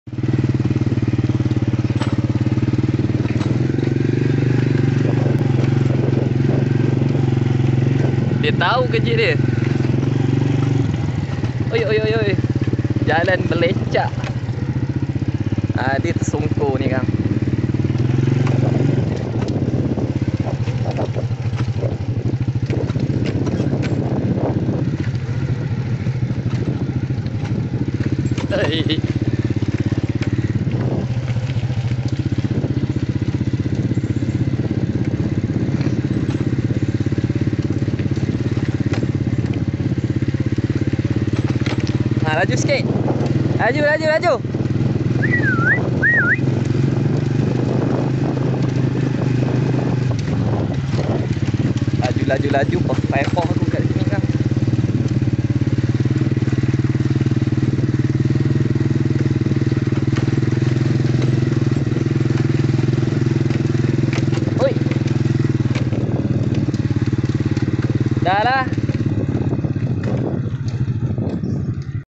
Honda EX5 Offroad Track Ride With 6 months old dog | Big Boy Blackie know his territory.
Part of his endurance training 🐕💨

Ride Honda EX5 keluar kebun nak pi breakfast .